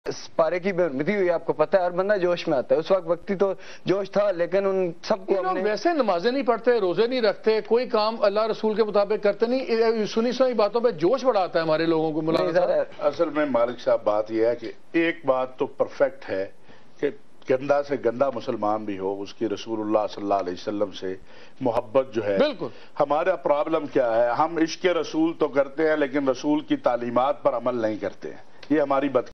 Question of the century on Blasphemy by Mohammad Malick
Mohammad Malick is Reporter, Anchor of Dunya @ 8, Group Editor English Publications at Dunya News.